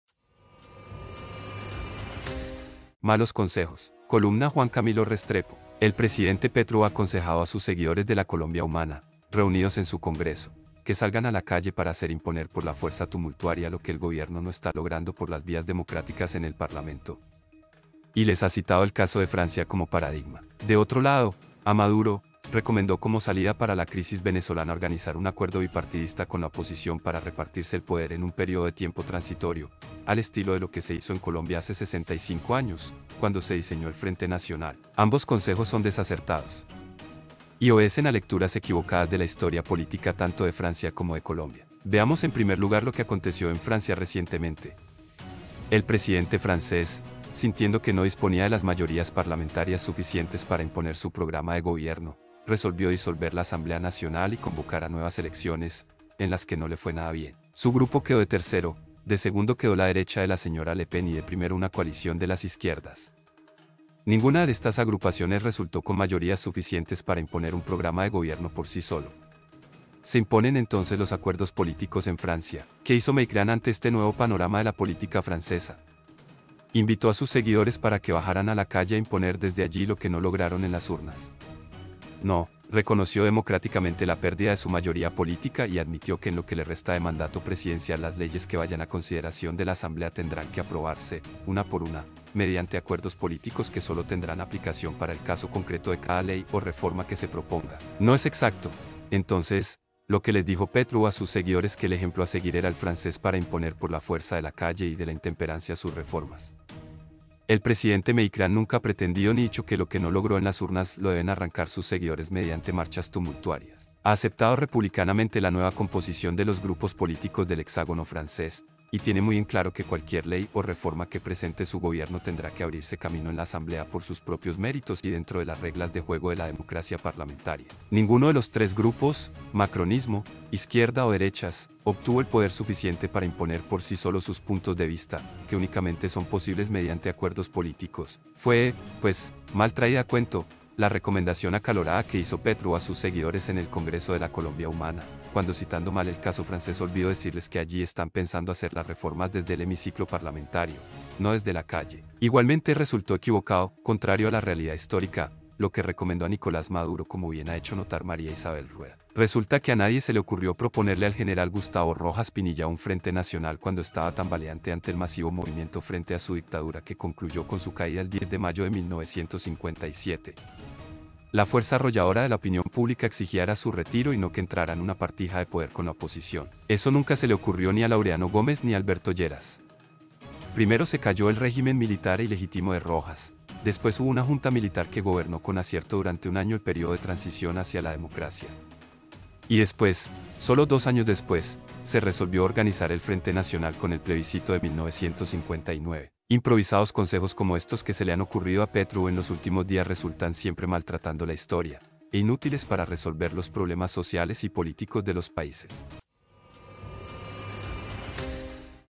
MALOS CONSEJOS 📢 (El presidente Petro ha aconsejado a sus seguidores de la Colombia Humana...)
El presidente Petro ha aconsejado a sus seguidores de la Colombia Humana, reunidos en su congreso, que salgan a la calle para hacer imponer por la fuerza tumultuaria lo que el gobierno no está logrando por las vías democráticas en el Parlamento. Y les ha citado el caso de Francia como paradigma.